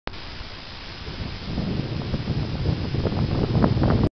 Oyster catcher near Tan Lan, Llanrwst
Oyster catcher near Tan Lan, Llanrwst - This is taken on April the 27th 2010. It is an oyster catcher flying over the Conwy River at Tan Lan, near Llanrwst.